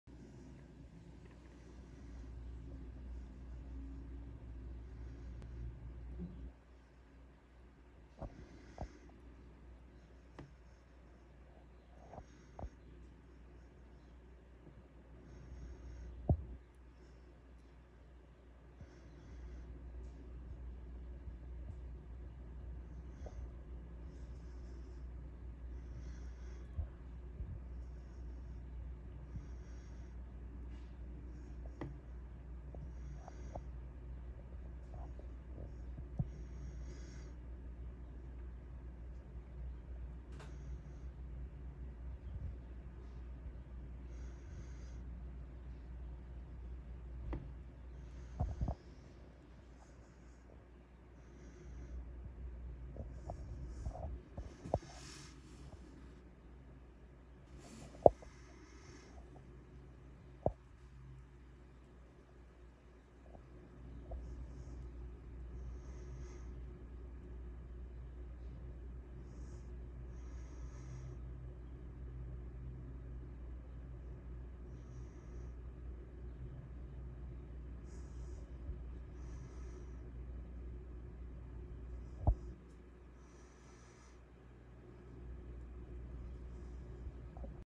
Monarch caterpillar
Caterpillar searches terrarium to find the perfect spot to form its chrysalis